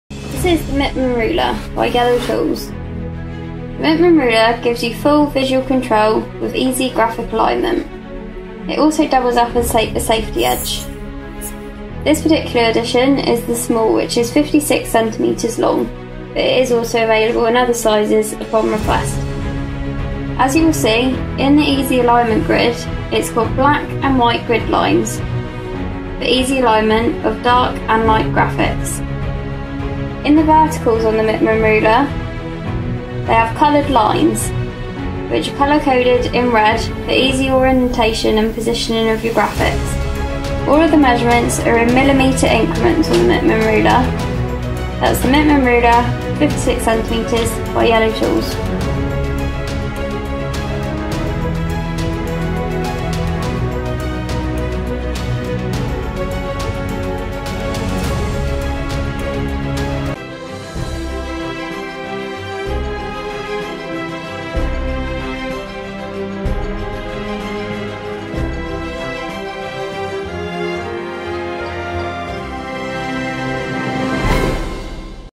SIGNGEER Mittman Ruler by YelloTools
The ruler which gives you full visual control for easy alignment of your graphics - works with both dark and light colouring thanks to the easy view black and white guide lines! Its vertical line is colour coded to enable your perfect orientation and positioning of the ruler; in millimetre increments.